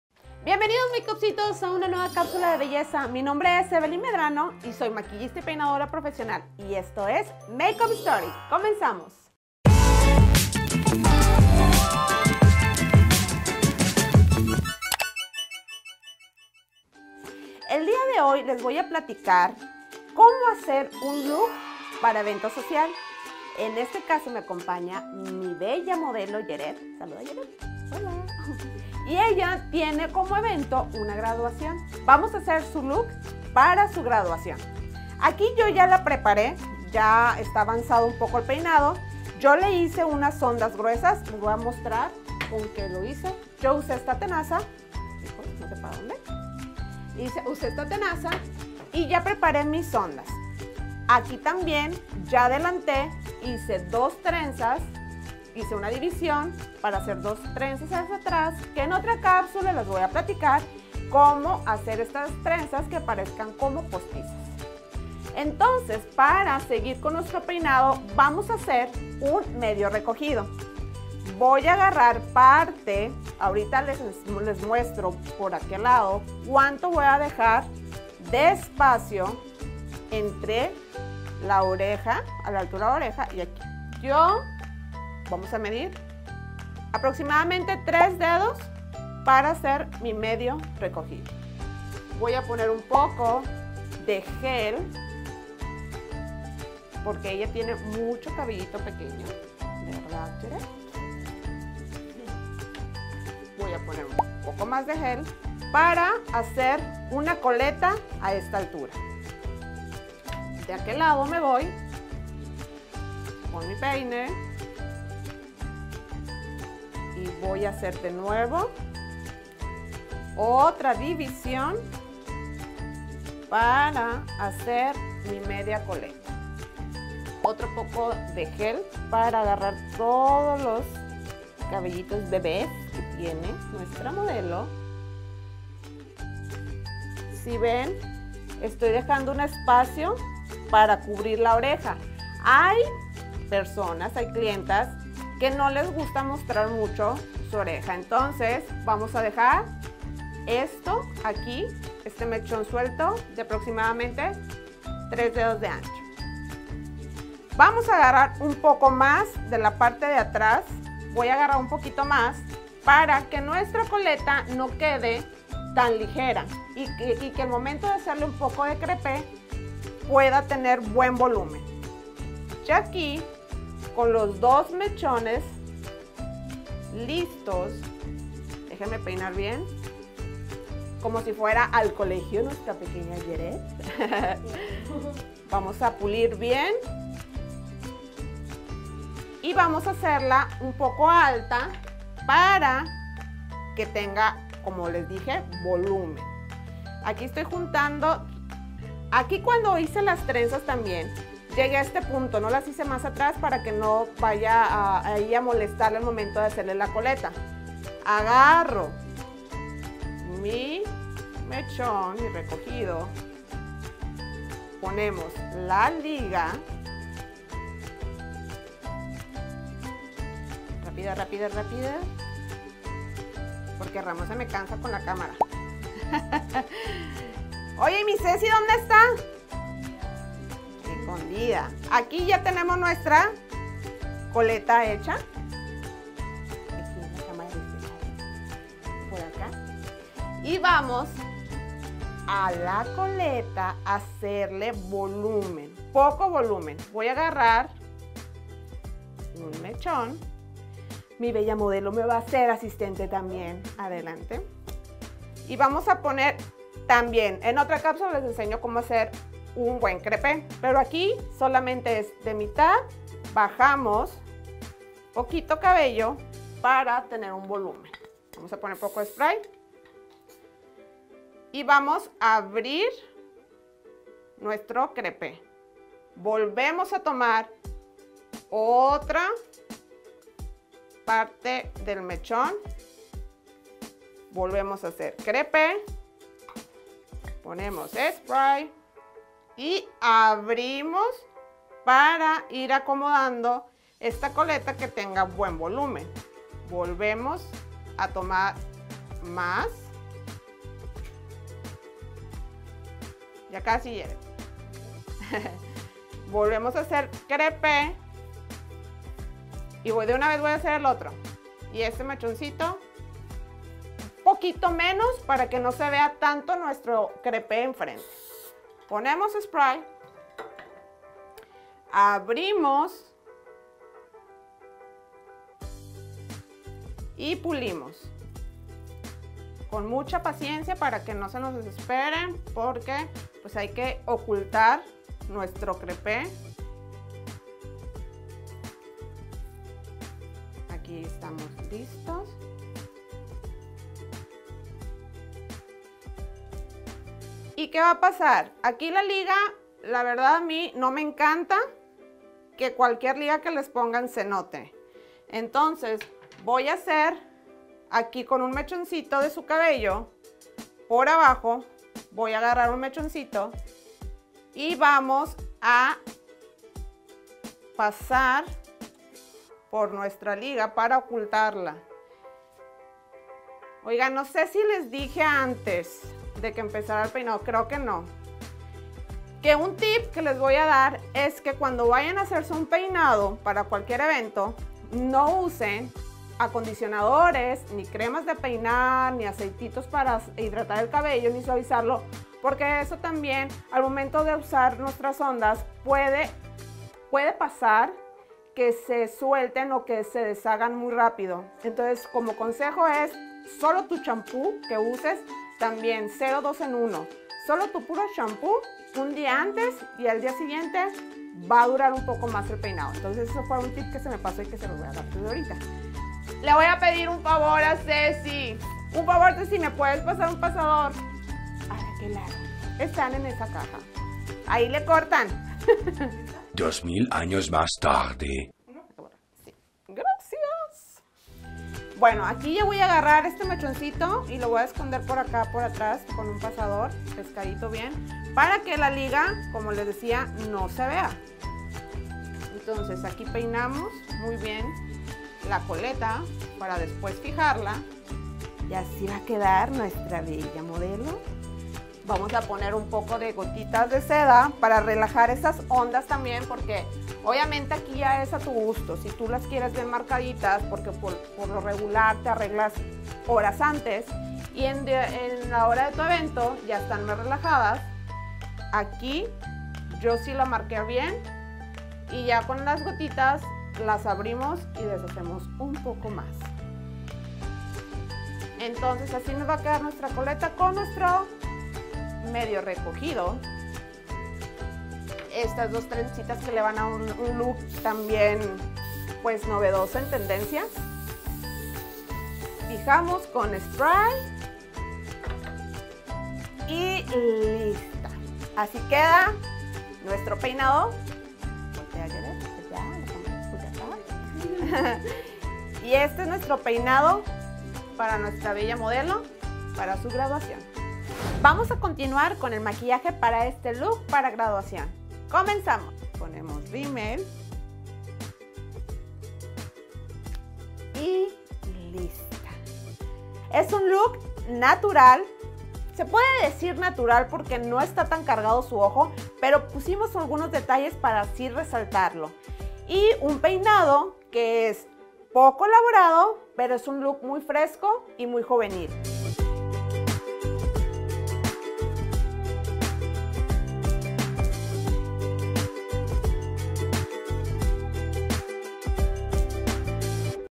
Peinado con cabello suelto para un evento social 🥳- Makeup Story
¡Hola Makeupcitos! Para este video haremos un peinado para graduación con cabello suelto 🥳🤩